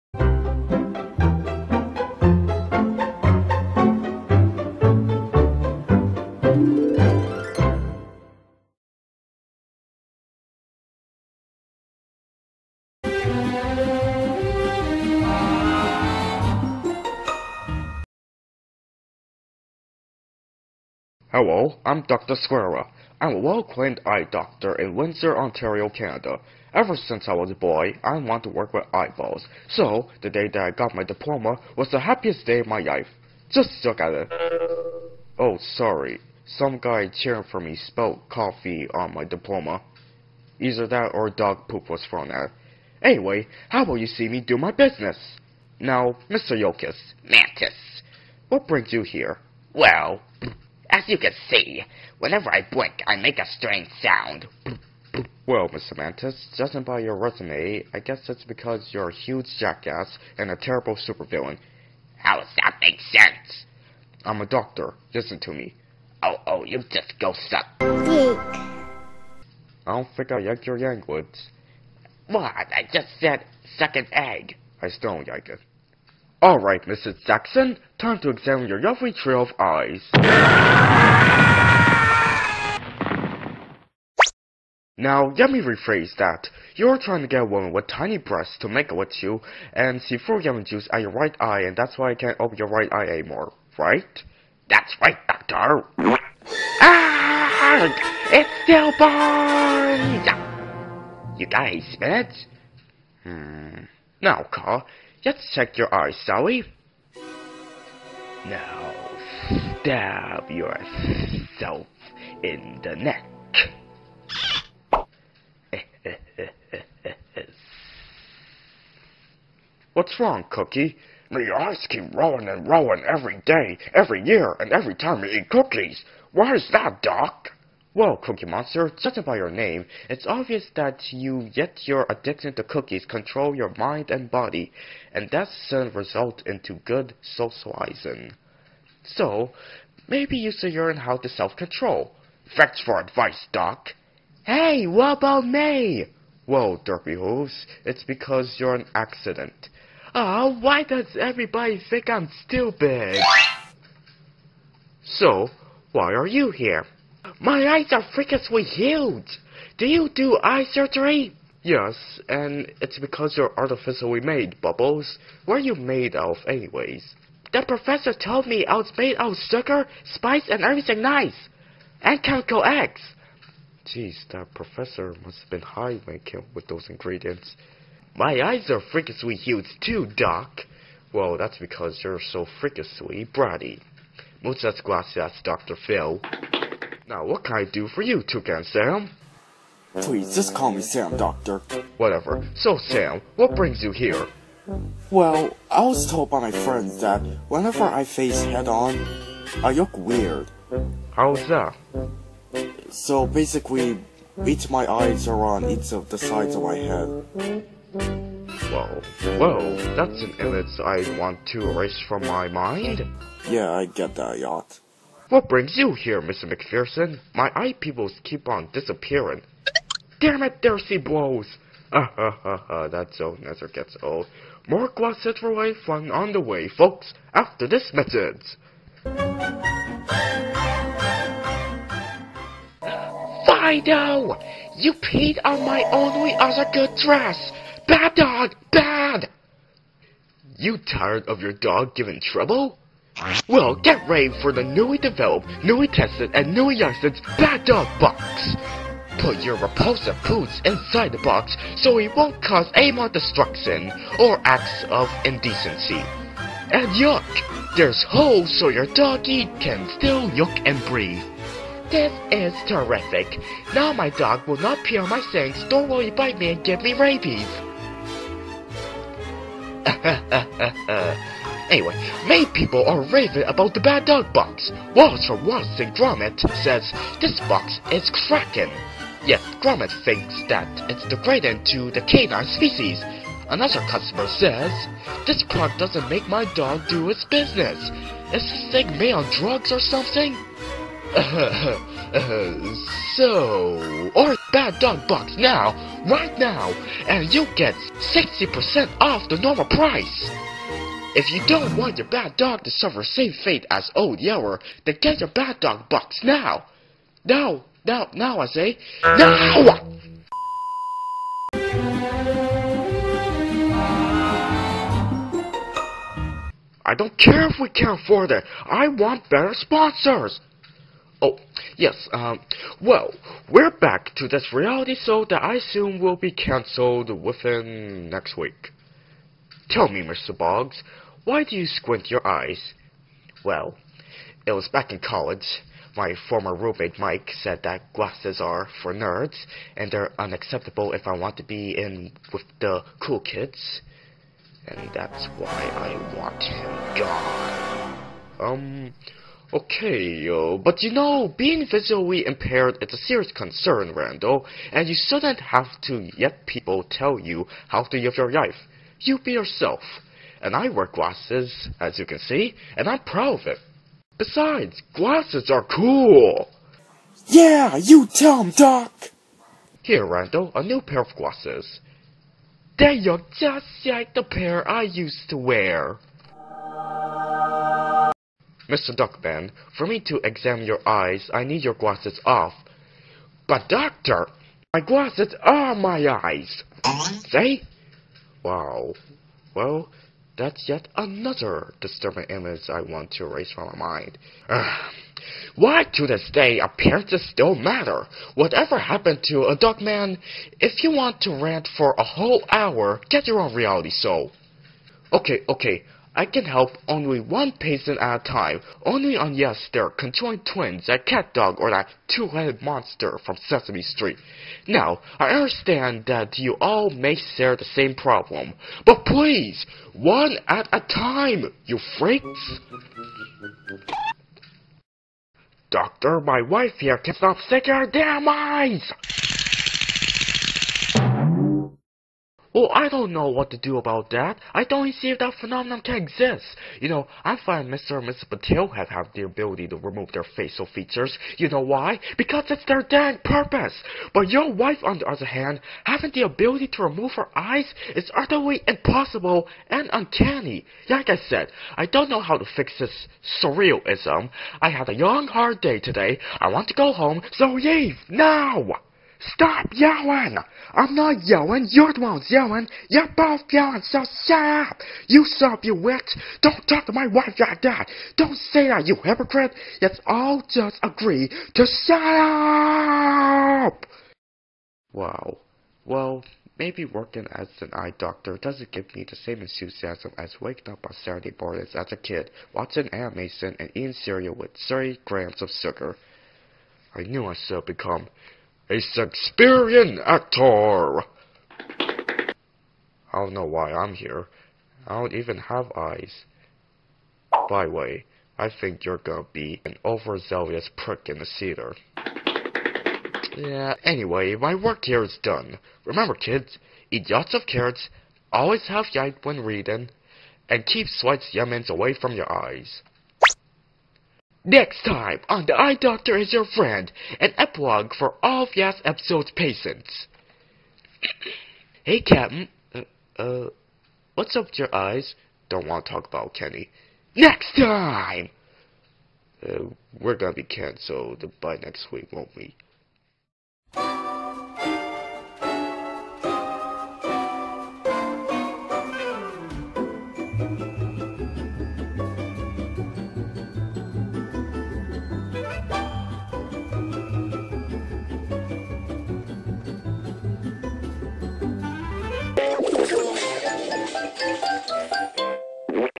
The Eye Doctor is Your Friend!
My second animated short made using Paint and Windows Movie Maker is also my first one with a story of sorts. It's a fake reality show titled "The Eye Doctor is Your Friend!", where Dr. Sclera, a self-described "well-acclaimed eye doctor in Windsor, Ontario, Canada," checks on and gives advice to different cartoon characters (plus one Muppet).

They include, in order of appearance:
Zorak (Space Ghost Coast to Coast)
Naomi Jackson (Monsters University)
Popeye the Sailor
Kaa (The Jungle Book)
Cookie Monster (Sesame Street), and Derpy Hooves (My Little Pony: Friendship is Magic)
Bubbles (The Powerpuff Girls)
Bart Simpson (The Simpsons)
Toucan Sam (Froot Loops cereal mascot, in his stuffed animal form from my series "Nigel and Friends")
Darryl MacPherson (Baby Blues)
Randall Boggs (Monsters, Inc./Monsters University)
Duckman
Leela (Futurama), Mike Wazowski and Celia (Monsters, Inc.), Plankton (SpongeBob SquarePants), Kurt Blobberts (Lloyd in Space), Jackie Khones (Foster's Home for Imaginary Friends), and Commander Peepers (Wander over Yonder)
The couple from the Oscar-winning short "The Big Snit"
Dogbert (Dilbert)

I've been working hard on this satirical, limited-animated crossover short between July and November of 2014. Also included in-between is a funny fake ad for a fake product, Bad Dog Box! Say whatever negative thing you want about the intentionally cheap animation, which, to self-deprecate, makes "Space Ghost Coast to Coast" look like Don Bluth. But remember that I'm still an amateur, and that as long as the story, humor, and
acting is okay, the not-so-fluid animation shouldn't be too much of a distraction.

This may be my best animated short, so far. Enjoy! Oh, and coming this holiday season, an immediate short sequel featuring Ginger and Dodie from "As Told by Ginger"! Love that show!